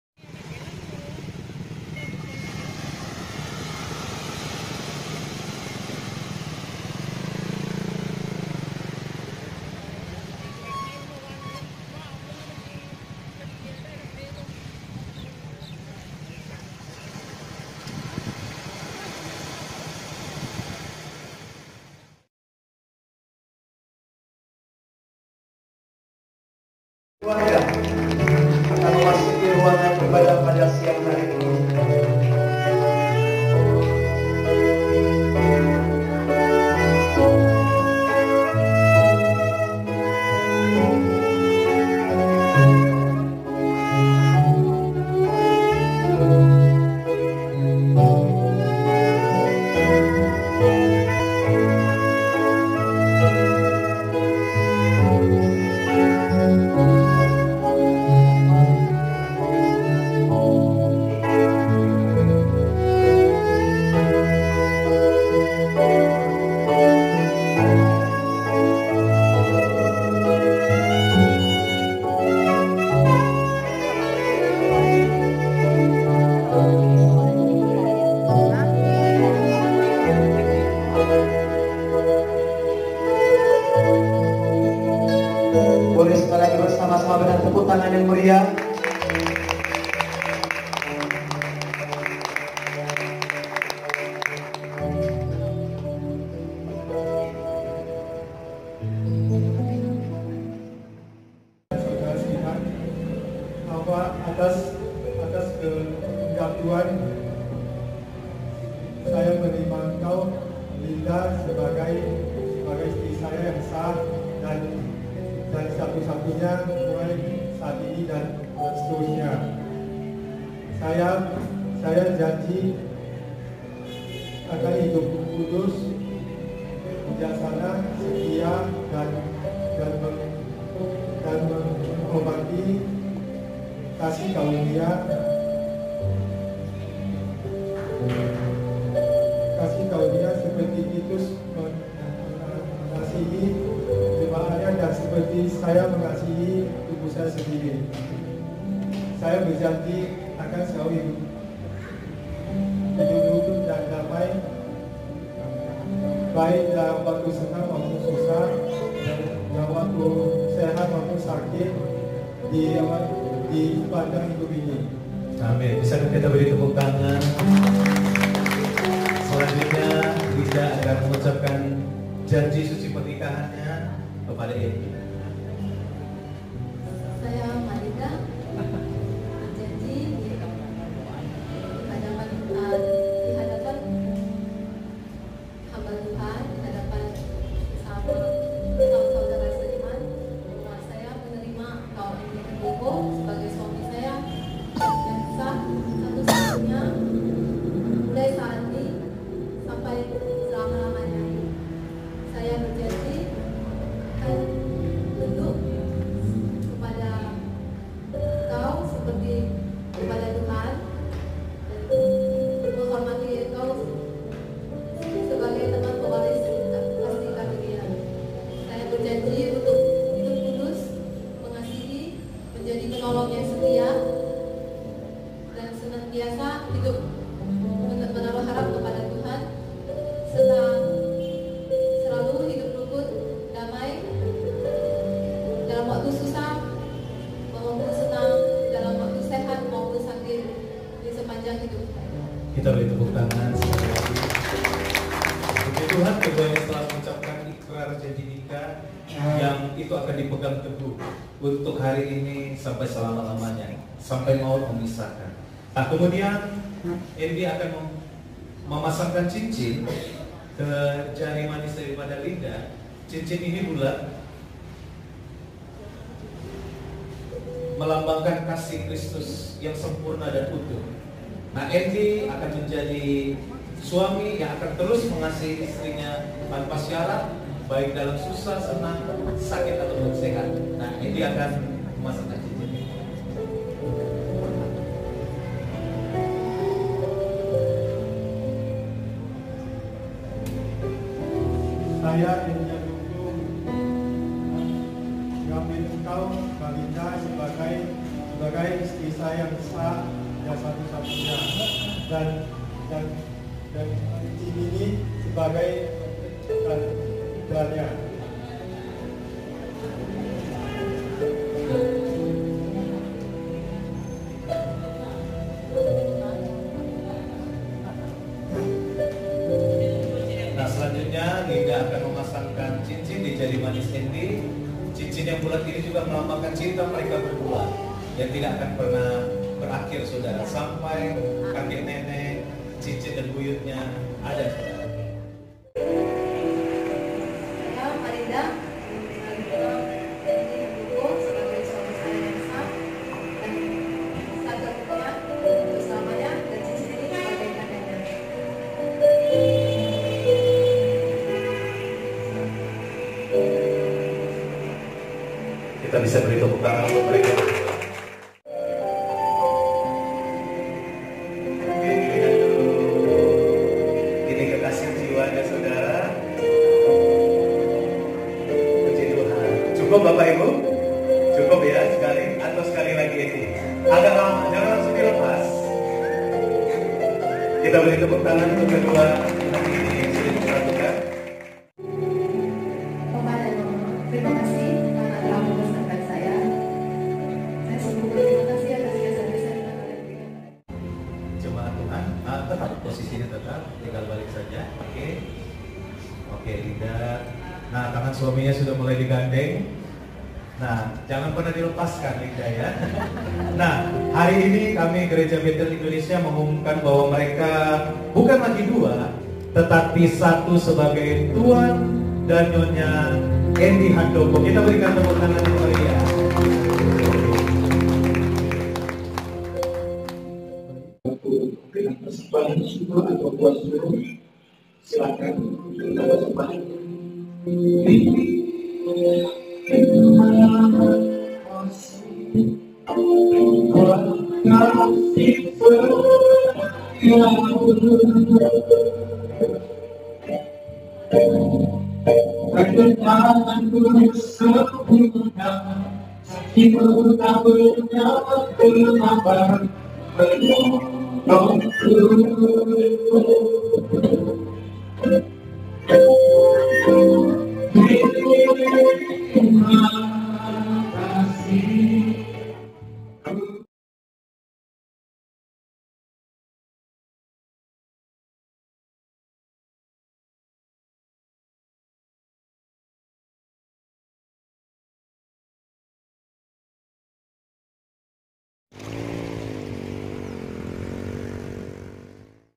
Pemberkatan Nikah GBI (Holy Matrimony Gbi Church)
Banyak yg penasaran seperti apakah Pemberkatan Nikah Gereja Bethel Indonesia/GBI?
Sebelum menikah, saya sendiri sangat penasan seperti apa ya?
Saya search Google, tapi tdk dpt satupun gambarannya, Kali ini saya share video ini supaya teman teman seiman tahu jadwalnya seperti apa ya.
Video ini adalah gambaran kasar alur pemberkatan, dan merupakan hasil jeperetan dan video family saya, Kalo mau video jelasnya silakan subscribe dan mentiom jika ingin saya post video jelasnya yg siap sebulan kemudian